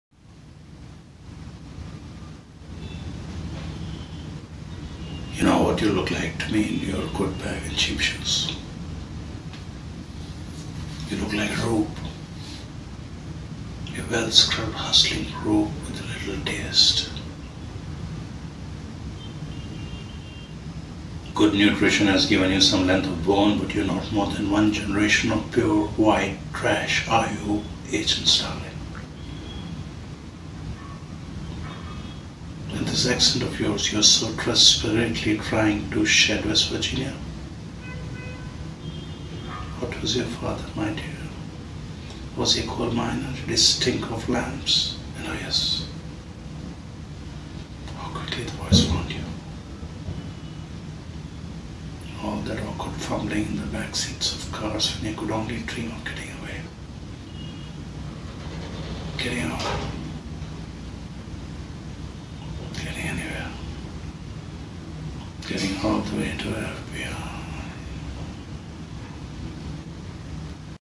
Monologue from SilenceOfThelambs
ActorRajeevGupta Playing HannibalLecter immortalized by Sir Anthony Hopkins